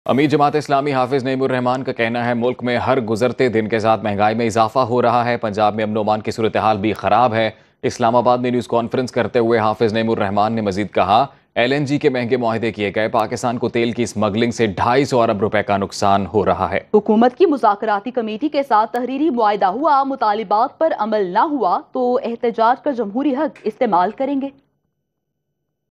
Hafiz Naeem Ki Khatarnak Dhamki | Govt in Trouble | Jamat e Islami Another Dharna | Breaking News
"Hafiz Naeem ki taraf se di gayi khatarnak dhamki ne sarkar ko pareshani mein daal diya hai! Jamat e Islami ne ek aur dharna ka elan kiya hai. Is khabar ka kya asar hoga? Janiye tamam tafseelat is Breaking News segment mein sirf Suno News HD par. Dekhiye kaise siyasat ka maidan garam hota ja raha hai!"